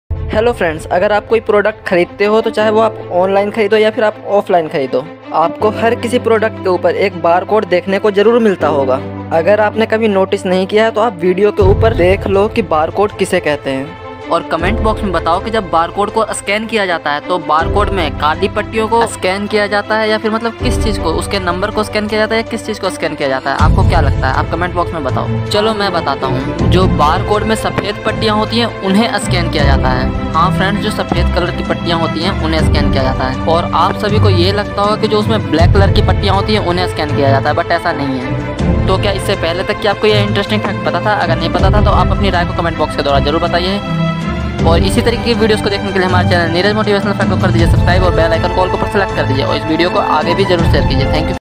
QR code and barcode ki jankari| QR code and barcode kya hai| Neeraj motivational fact hindi
Hello dear,
Friends is video me hamne da code ke bare mein bataya hai.